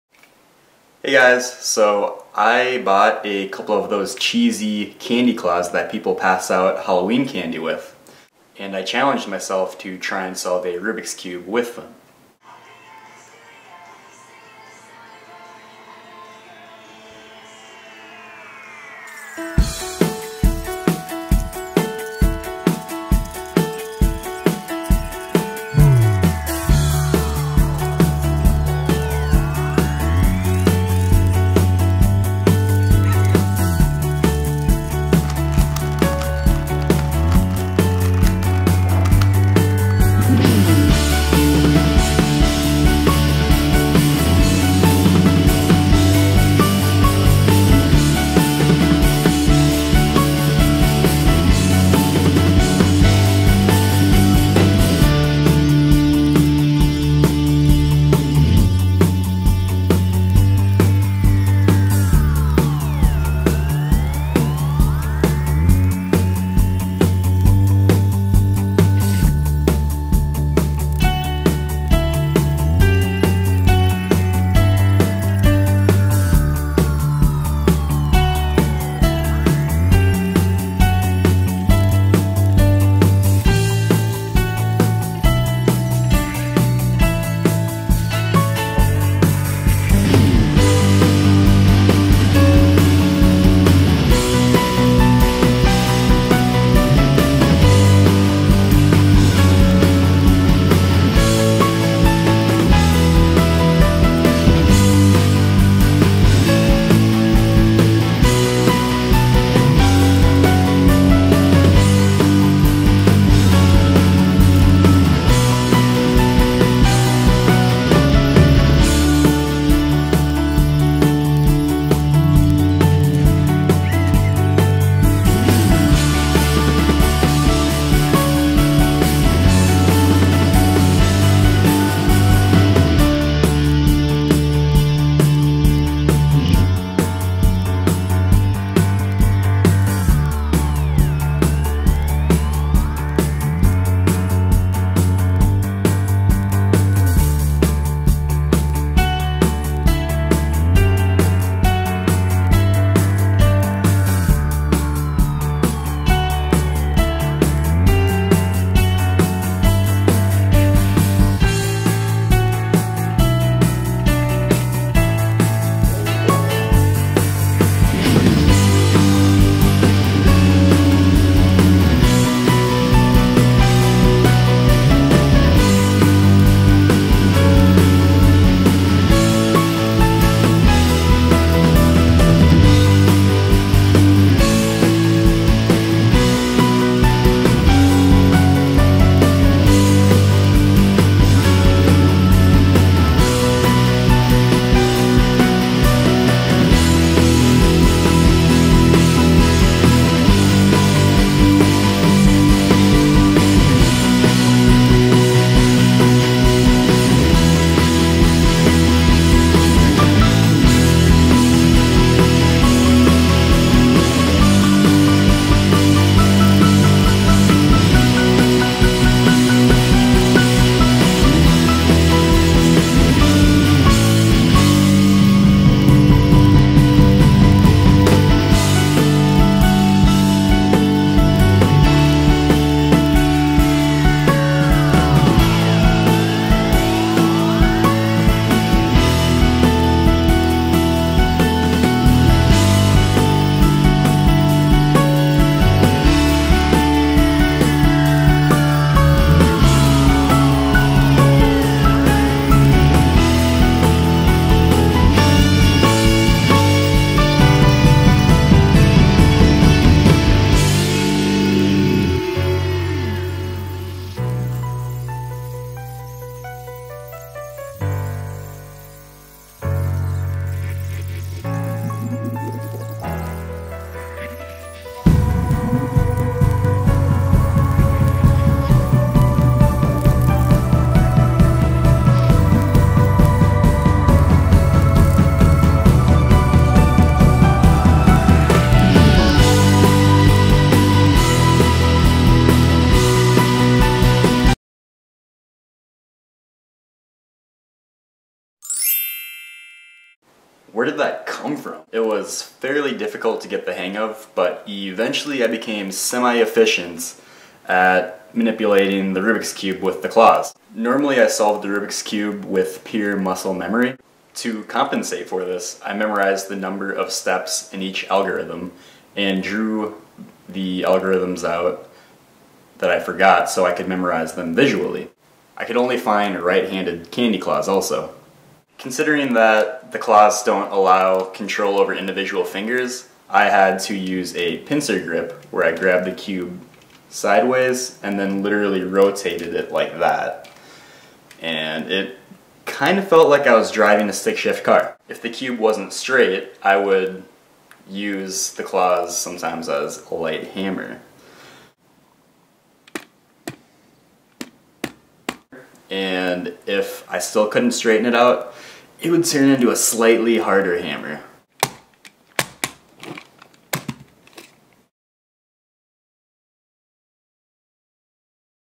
Guy solves 3X3 RUBIK'S CUBE with CANDY CLAWS
Me solving a 3x3 rubiks cube with those claws people pass out candy with on Halloween :)